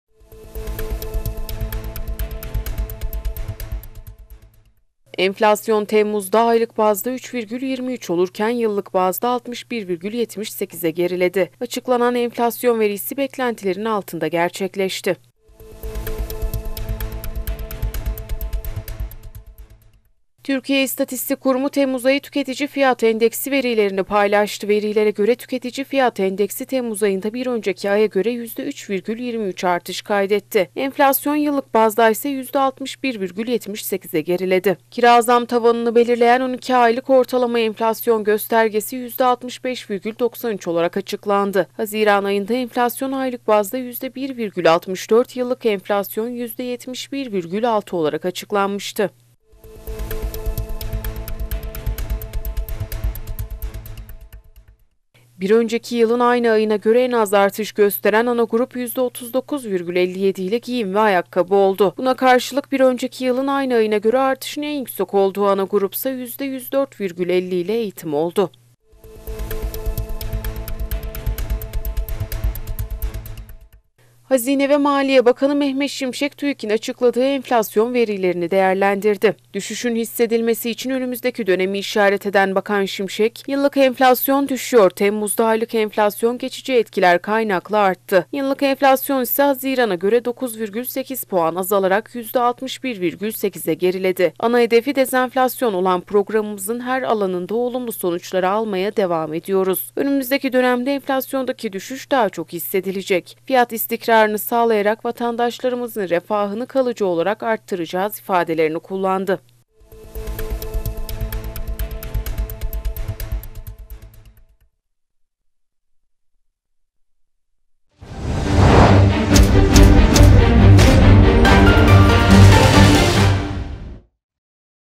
Kardelen TV I TEMMUZ AYI ENFLASYON RAKAMLARI AÇIKLANDI
Enflasyon, Temmuzda Aylık Bazda Yüzde 3,23 Olurken Yıllık Bazda Yüzde 61,78'e Geriledi. Açıklanan Enflasyon Verisi Beklentilerin Altında Gerçekleşti.